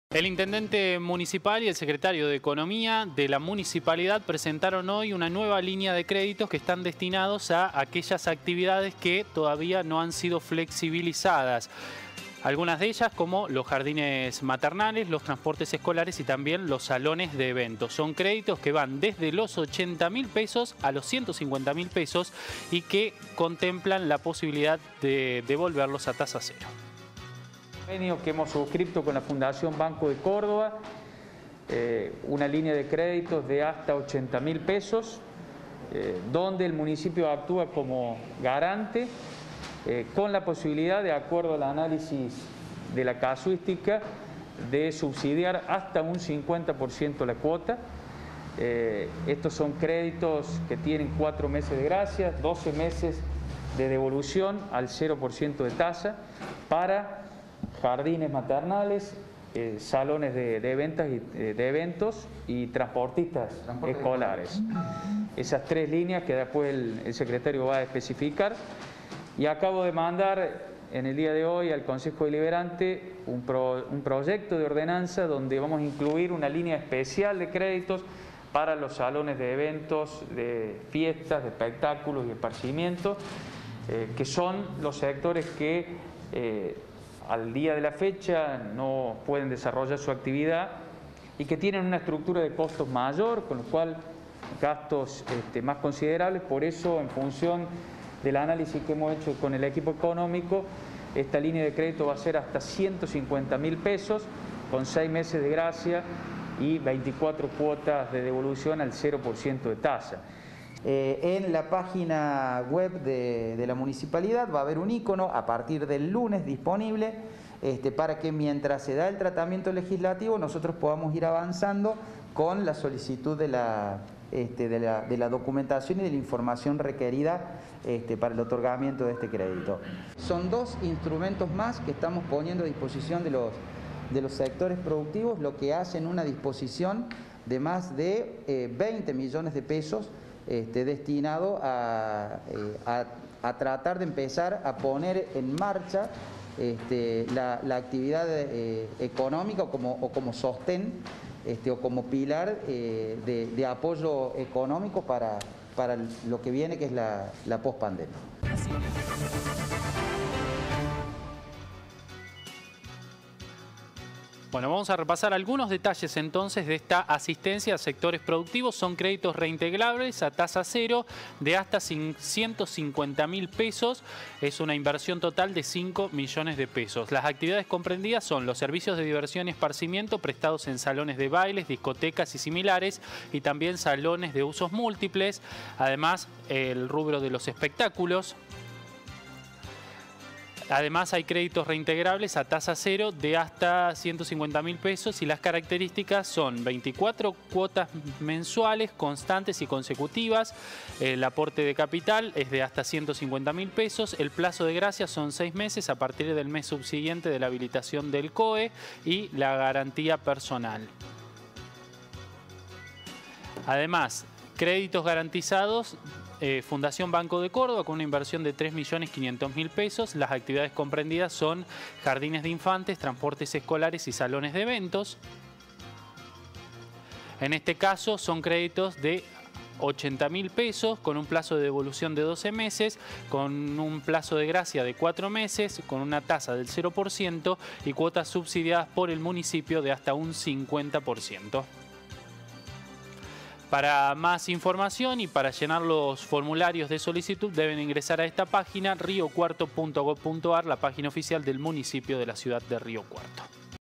La municipalidad lanza una nueva línea de créditos a tasa cero
La municipalidad lanza una nueva línea de créditos a tasa cero para actividades que aún no fueron flexibilizadas: CRÉDITOS REINTEGRABLES A TASA 0% DE HASTA $150.000,- Inversión $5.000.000 ACTIVIDADES COMPRENDIDAS Servicios de diversión y esparcimiento prestados en salones de bailes, discotecas y similares y salones de uso múltiple. Espectáculos.CARACTERÍSTICAS - 24 Cuotas mensuales, constantes y consecutivas - Capital: Hasta $150.000.- - Plazo de Gracias: 6 meses a partir del mes subsiguiente de la habilitación del COE - Garantía Personal.CRÉDITOS GARANTIZADOS FUNDACIÓN BANCO DE CÓRDOBA Inversión $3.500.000 ACTIVIDADES COMPRENDIDAS - Jardines de Infantes - Transportes Escolares - Salones de Eventos.CRÉDITOS GARANTIZADOS FUNDACIÓN BANCO DE CÓRDOBA CARACTERÍSTICAS - Capital: $80.000.- - Plazo de devolución: 12 meses - Plazo de Gracias: 4 meses - Tasa: 0% - Cuotas subsidiada por le Municipio hasta un 50%ENCONTRÁ EL FORMULARIO SOLICITUD